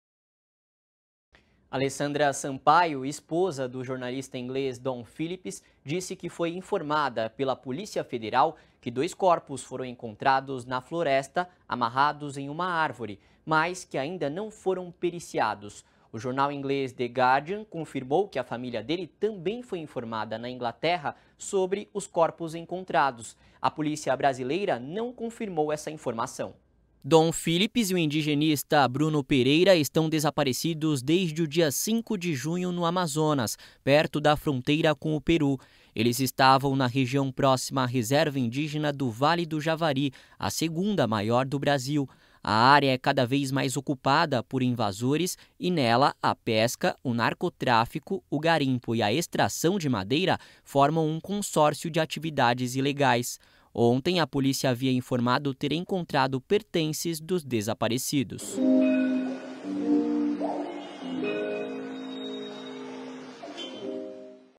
Esposa de jornalista desaparecido no Amazonas diz que polícia encontrou corpos
Alessandra Sampaio, esposa de Dom Phillips, afirmou que foi informada pela polícia que dois corpos foram encontrados amarrados em uma árvore e que estão sendo periciados. Dom Phillips e o indigenista Bruno Pereira estão desaparecidos há mais de uma semana.